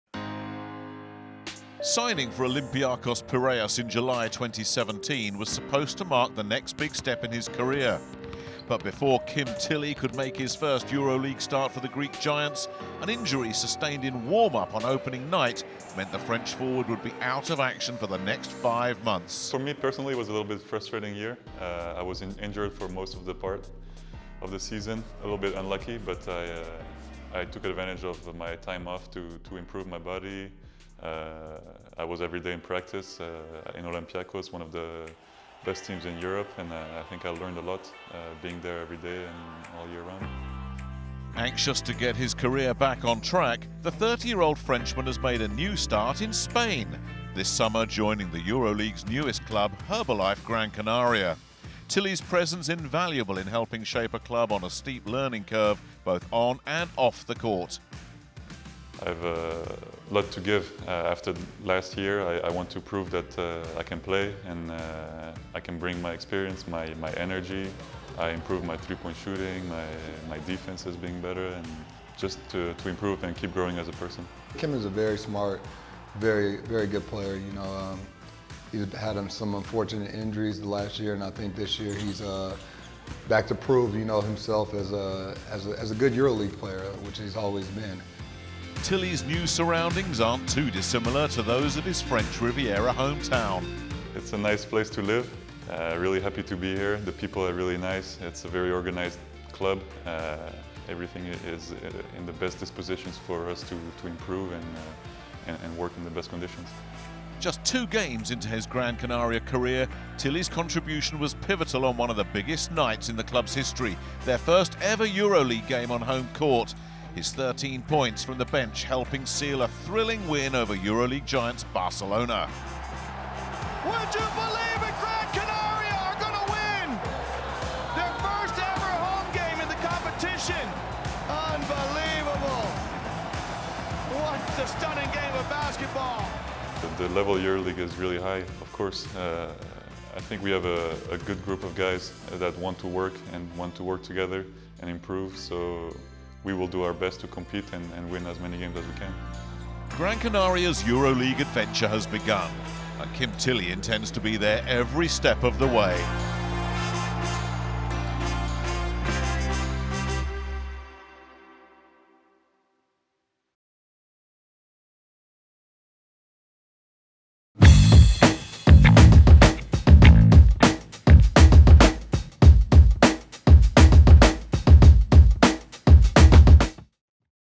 Focus on: Kim Tillie, Herbalife Gran Canaria
Kim Tillie went back to Spain last summer after spending a season in Greece with Olympiacos Piraeus, in which he had to deal with a serious injury. Now, he is in Gran Canaria to return to the EuroLeague.